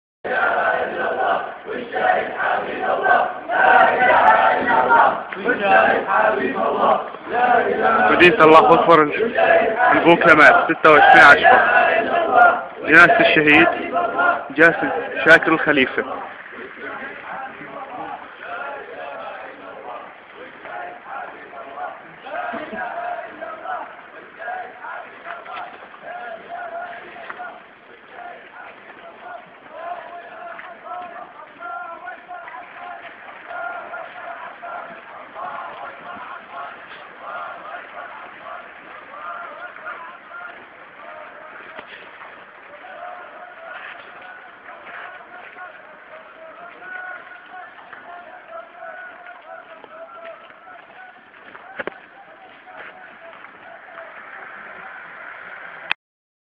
Funeral of Martyr Jassim Al-Khalifah in Abu Kamal City, Deir Ezzor, 26-Oct-11
ديربرس - تشييع الشهيد جاسم الخليفة في البوكمال 26-10